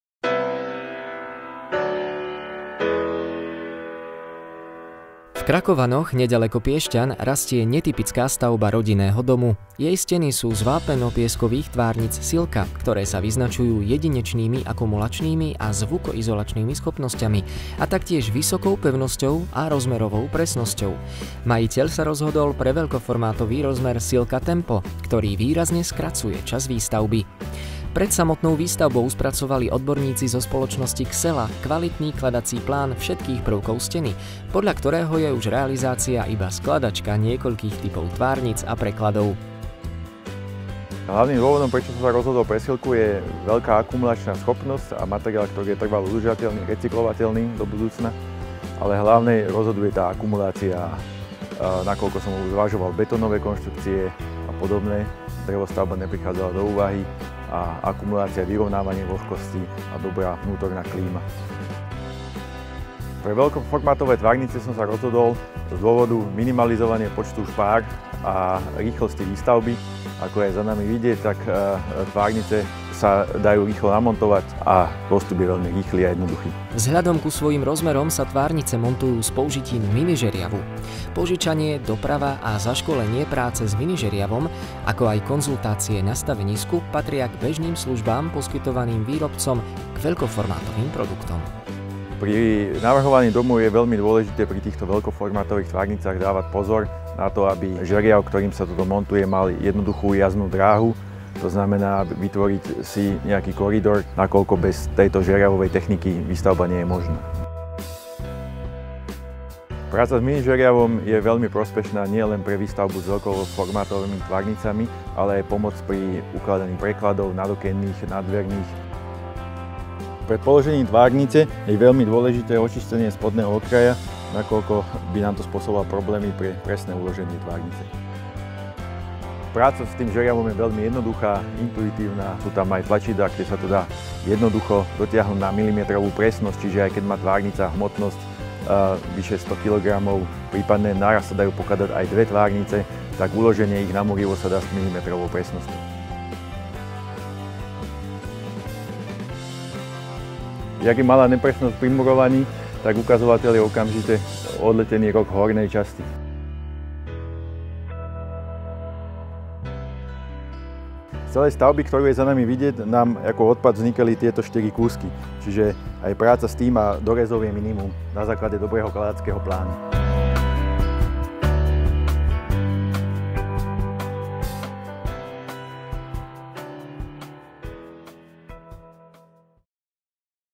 Rodinný dom z tvárnic Silka Tempo | Ytong
V Krakovanoch rastie netypická stavba rodinného domu. Jej steny sú z vápenno-pieskových tvárnic Silka, ktoré sa vyznačujú jedinečnými akumulačnými a zvukoizolačnými schopnosťami. Majitel se rozhodl pro veľkoformátový rozmer Silka Tempo, ktorý výrazne skracuje čas výstavby.